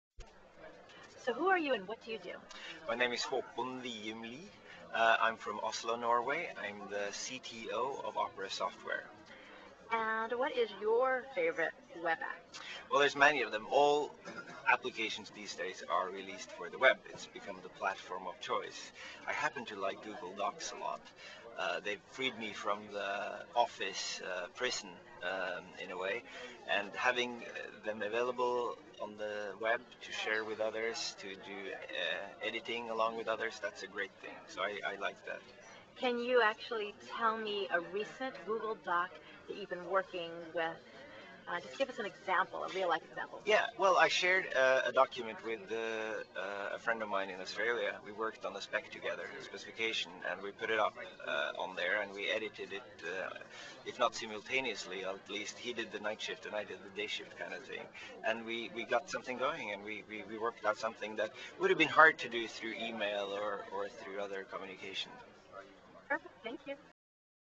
What is Your Favorite Web App, Håkon Wium Lie?
Håkon Wium Lie from Opera discusses his favorite Web app with WebWorkerDaily.